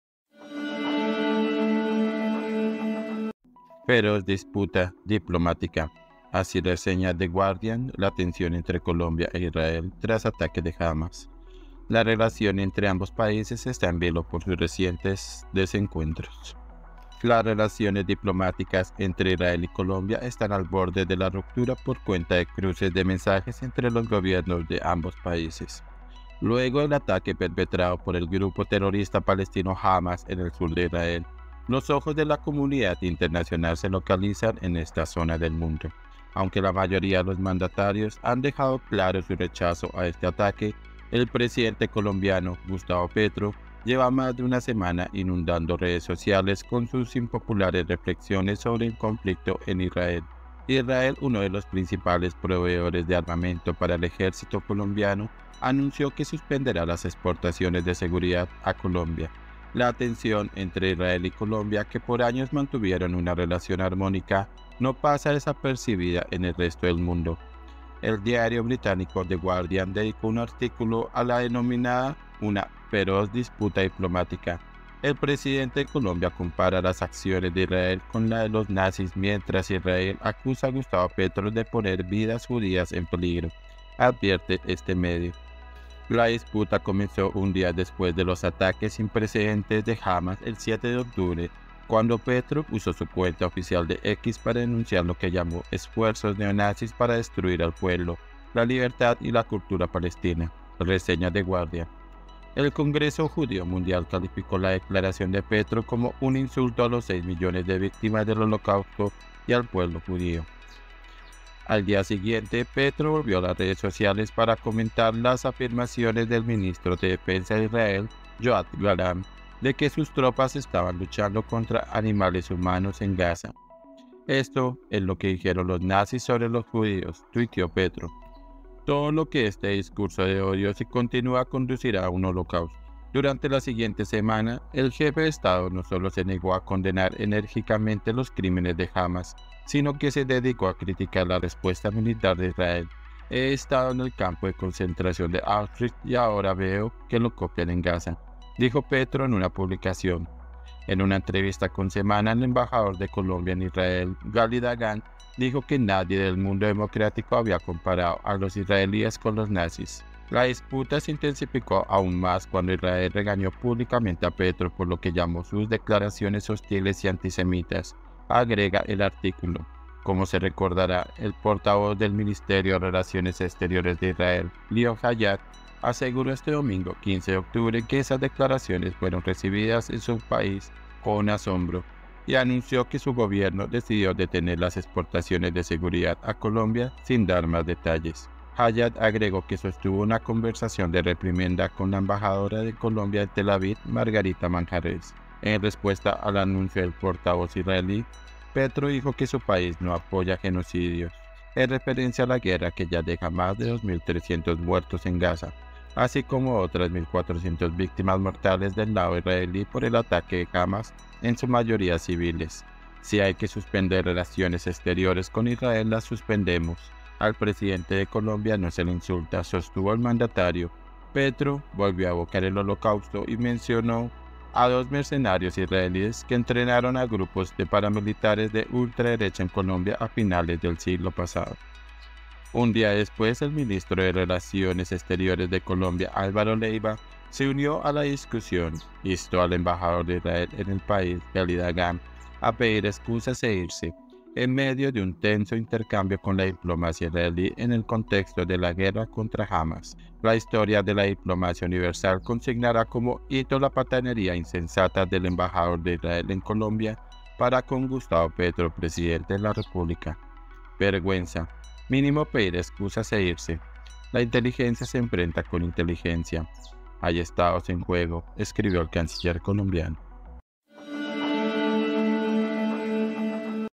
"FEROZ DISPUTA DIPLOMÁTICA" 📰 así reseña The Guardian la tensión entre Colombia e Israel
La tensión entre Israel y Colombia, que por años mantuvieron una relación armónica, no pasa desapercibida en el resto del mundo. El diario británico The Guardian dedicó un artículo a la que denomina una “feroz disputa diplomática”.
FUENTE EL PAÍS DE CALI